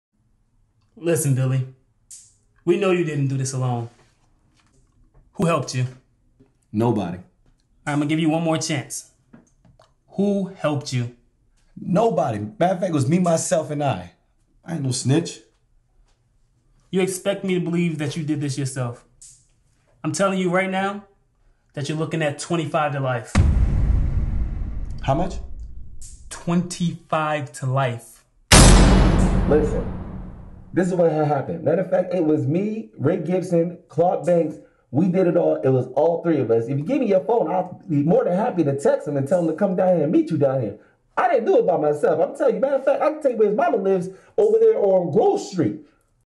You didn’t hear this from me
Comedy video for people that snitches on people for crimes